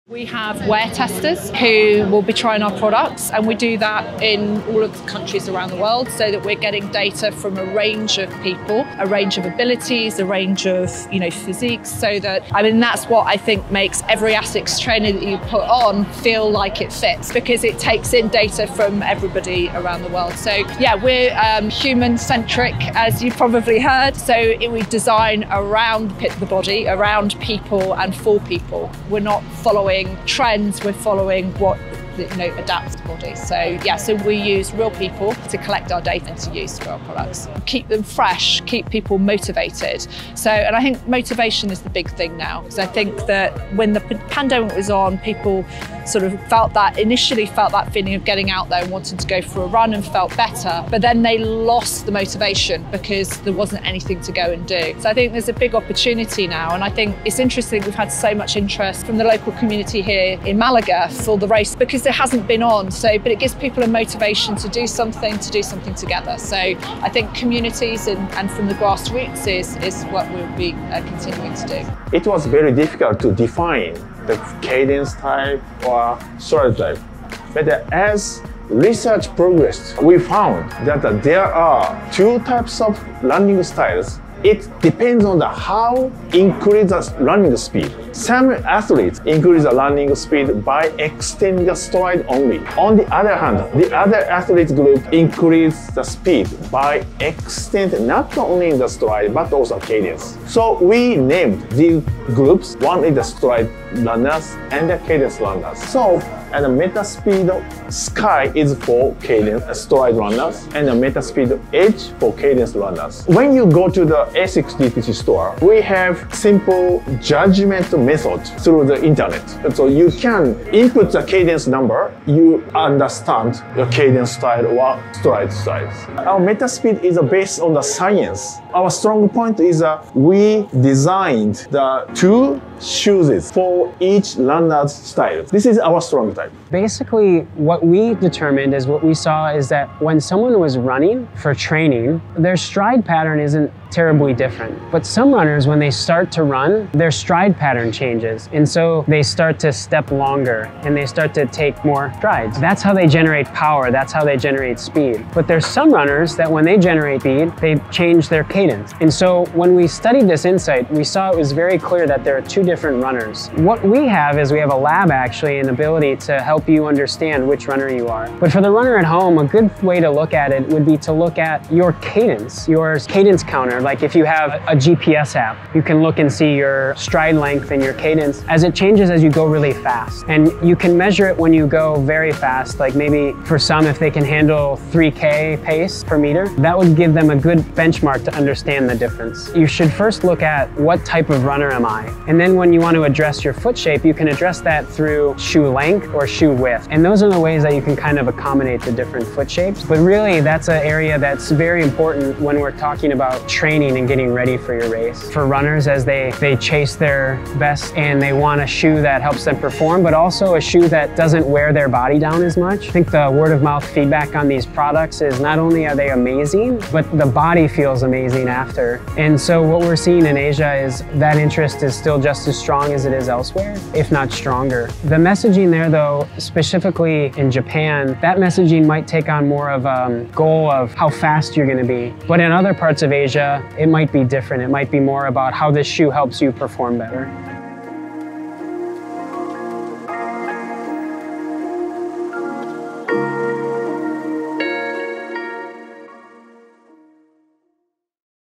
BRAINS BEHIND THE ASICS METASPEED+
The ASICS METASPEED+ Series Is In Malaysia!

ASICS has utilized data accumulated through its elite athletes to refine the product design to cater to the two major running styles allowing every runner to perform at their personal best.

In this video, you will be getting first-hand knowledge from three key persons of the METASPEED Sky+ and Edge+ shoes at the ASICS Innovation Summit 2022.